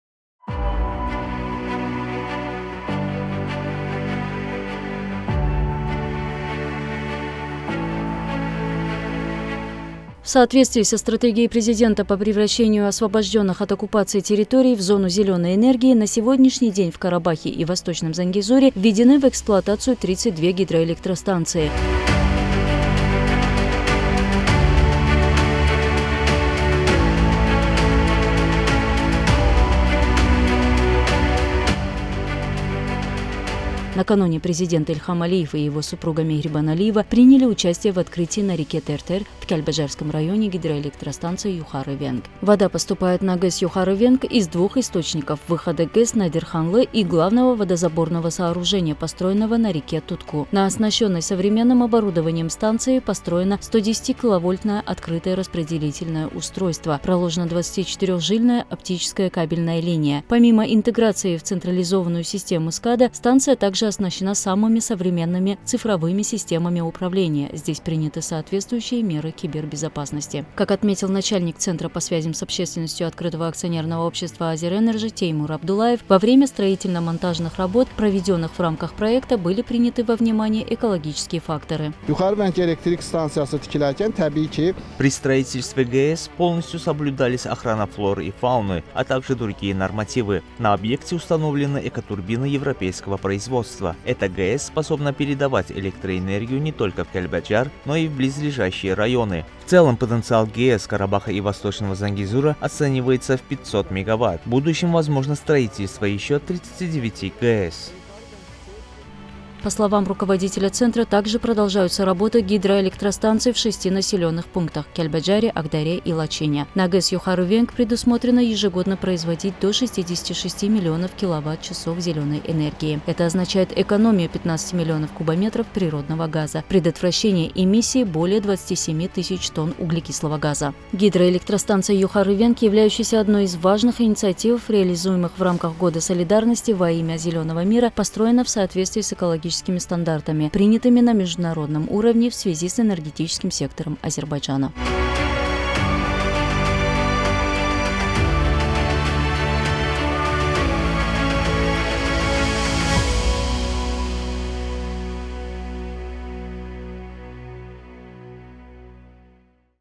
На ГЭС «Юхары Венг» ежегодно будет производиться до 66 млн кВт-ч зеленой энергии
В соответствии со стратегией Президента Ильхама Алиева по превращению освобожденных от оккупации территорий в зону зеленой энергии на сегодняшний день в Карабахе и Восточном Зангезуре введены в эксплуатацию 32 гидроэлектростанции. Построенная в Кяльбаджаре гидроэлектростанция «Юхары Венг» является крупнейшим источником зеленой энергии, созданным в регионе после победы в Отечественной войне. Зеленая энергия, производимая на этой ГЭС мощностью 22,5 МВт, может полностью обеспечить потребность Кяльбаджарского района в электроэнергии на последующие 5 лет.